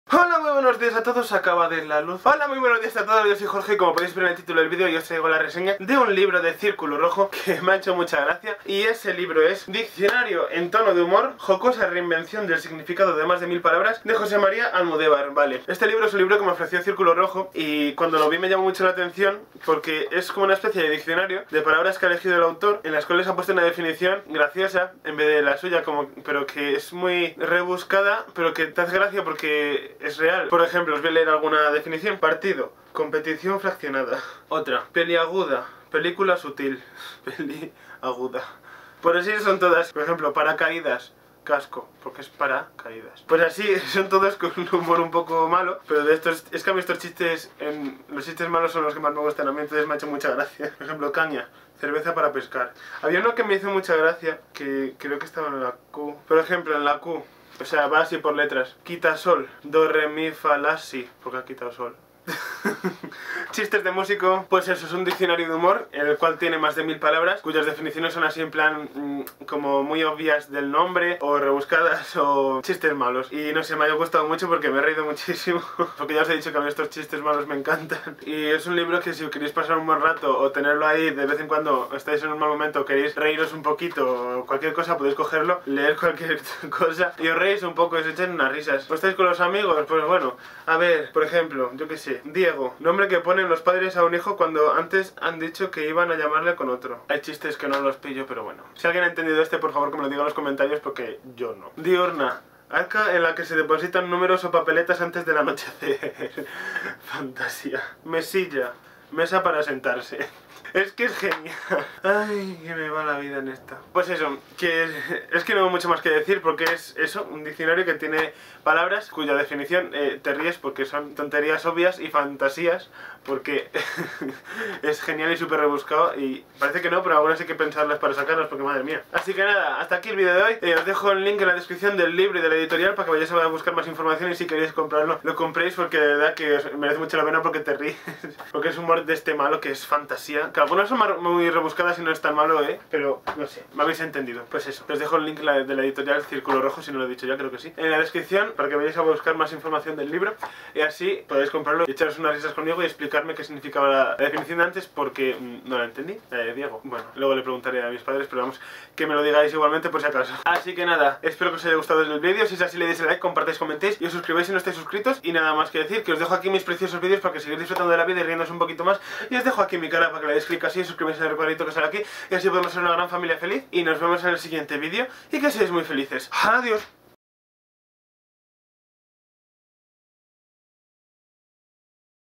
"Diccionario en tono de humor" de José María Almudévar
En Diccionario en tono de humor el autor reinventa el significado de más de 1200 palabras existentes en la lengua española. Observa cada una de ellas desde la perspectiva que más llama su atención, para terminar dándole una personal definición que, en muchas ocasiones, al lector se le antoja —además de divertida— más lógica que la oficial.